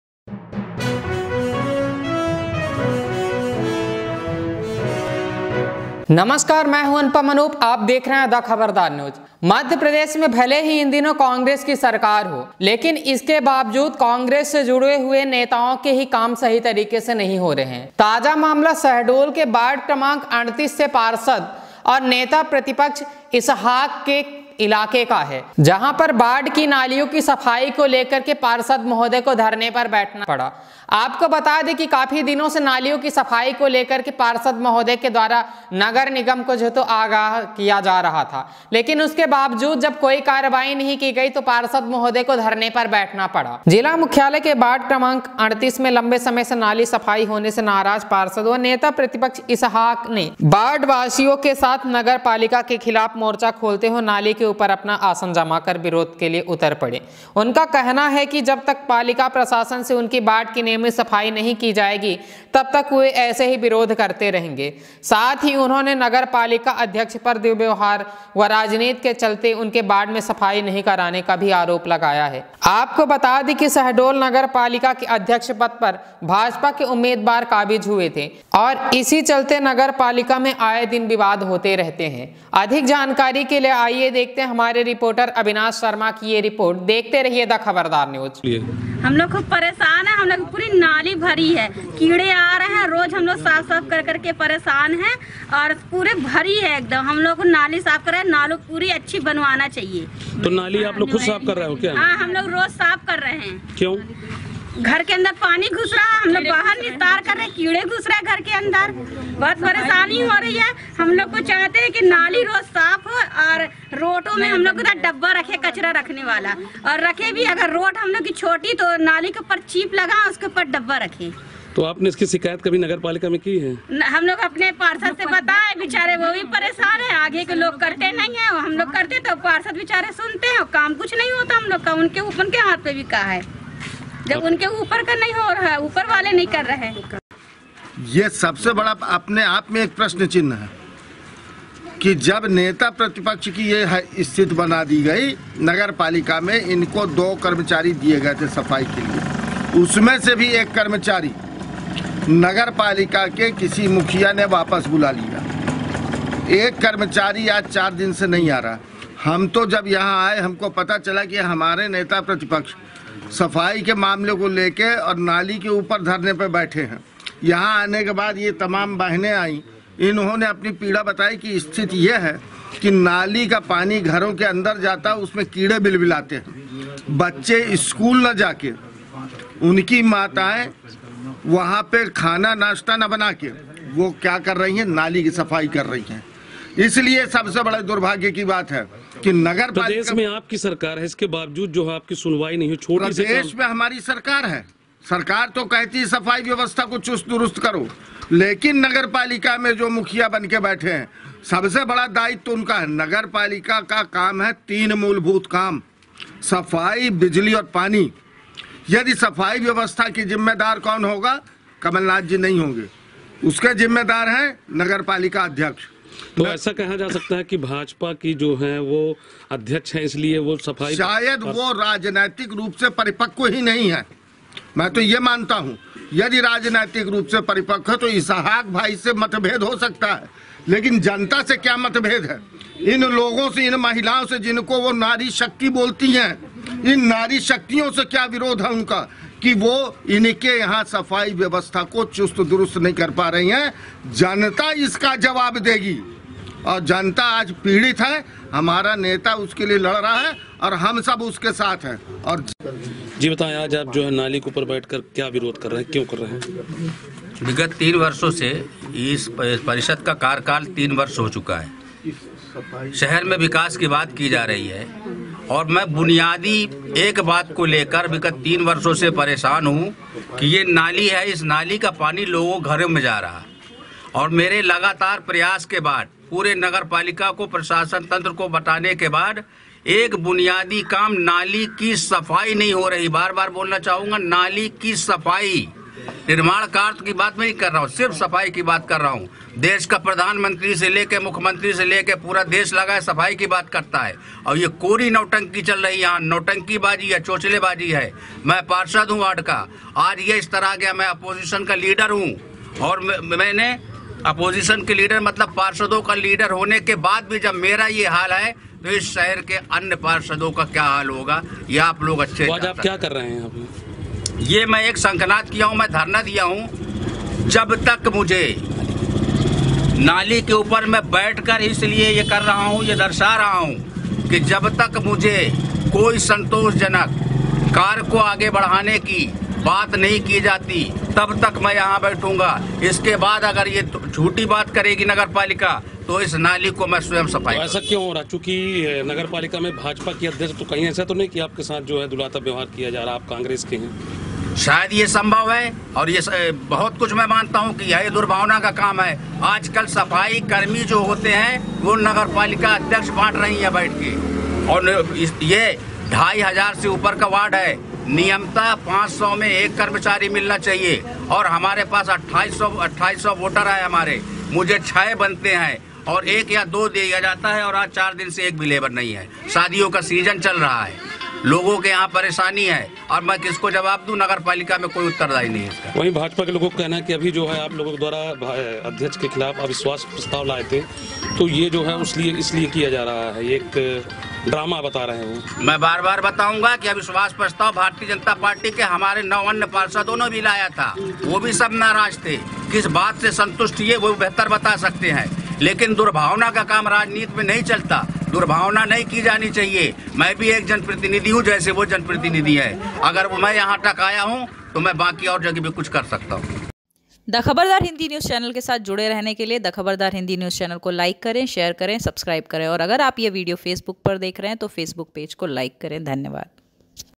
शहडोल कांग्रेसी पार्षद ने दिया निगम प्रशासन के खिलाफ अनोखा धरना,आप भी देखे..
शहडोल प्रदेश में कांग्रेश की सरकार होने  बाबजूद काँग्रेशी पार्षद को नाली साफ करने के लिए नाली में ऊपर बैठ विरोध करना पड़ा इससे बड़ा दुर्भाग्य कांग्रेश के लिए और क्या हो सकता है ।  जिला मुख्यालय के वार्ड नं 38 में लंबे समय से नाली सफाई नही होने से नाराज पार्षद व नेता प्रतिपक्ष इसहाक वार्डवासियों के आमर्थं से नगरपालिका के खिलाफ मोर्चा खोलते हुए नाली के उपर अपना आसन जमा विरोध पर उतर आए है।
उनका कहना है को जब तक पालिका प्रशासन से उनकी वार्ड की नियमित सफाई नही करेगी तब तक वे ऐसे ही विरोध करते रहंगे । साथ ही उन्होंने नगरपालिका अध्यक्ष पर दुर्व्यवहार व राजनीति के चलते उनके वार्ड में सफाई नही कराने का भी आरोप लगाया है ।

 गंदगी से भरी बजबजाती सड़ांध मार रही इस नाली के ऊपर आसन लगाए बैठे यह शख्स कोई और नही बल्कि वार्ड नं 38 का पार्षद व नगरपालिका शहडोल में नेता प्रतिपक्ष की भूमिका निभा रहे  इशहाक खान है । जो नगरपालिका की कारगुजारियों को लेकर नाराज है।   जिले मुख्यालय के नगरपालिका क्षेत्र पुरानी बस्ती वार्ड नं 38 में लंबे समय से नाली सफाई नही होने से वार्ड दुर्दसा है । वार्ड में नाली  सफाई नही होने से आलम यह है की नालियों में कचरे का अंबार लगा हुआ है ।
नालियां सड़ांध मार रही जिससे वार्डवासियों के रहना दूभर हो गया है । हालात ये है कि नाली  का गंदा पानी लोगो के घरों में घुस रहा । जिससे लोग नाराज है इसी बात का विरोध करते हुए पार्षद नाली के ऊपर बैठकर अपना विरोध जता रहे जिसका पूरे वर्डवासी समर्थन कर सड़क पर उतर आए है ।

  लंबे समय से वार्ड में नाली सफाई नही होने से परेशान  पार्षद समेत वर्डवासी इसकी मौखिक व  लिखित कई शिकायते  नगरपालिका कर चुके है । इसके बाबजूद उनके वार्ड में सफाई नही हो रही । जिससे नाराज वार्डवासियों के समर्थन से पार्षद ने अनोखे रूप से अपना विरोध प्रादर्शन किया ।

 आपको बता दे कि नगरपालिका शहडोल में भाजपा अध्यक्ष का कब्जा है । अभी हाल में ही भाजपा अध्यक्ष उर्मिला कटारे के खिलाफ काँग्रेशी पार्षदों के साथ साथ भाजपा व निर्दलीय पार्षदों ने अविश्वास प्रस्ताव लाया था । जिसके बाद पुनः वोटिंग के आधार पर वह एक बार फिर नपा अध्यक्ष के रूप में काबिज हुई । इसी बात को लेकर एक बार फिर राजनीति गरमाई है । वही विरोध कर रहे पार्षद इशहाक का कहना है कि राजनीति द्वेष के चलते उनके वार्ड में नाली सफाई नही कराई जा रही है ।

 नाली सफाई नही करने को  लेकर पार्षद द्वारा विरोध की जानकारी जैसे ही नपा अध्यक्ष व सीएमओ को लगी , वैसे ही वे अपने चेम्बर छोडकर भाग खड़े हुए, वही मीडिया नव भी  इस संबंध में  उनसे उनका पक्ष जानने के लिए संपर्क किया गया तो उन्होंने खुद अधिक व्यस्त बताते हुए मीडिया से पल्ला झाड़ लिया ।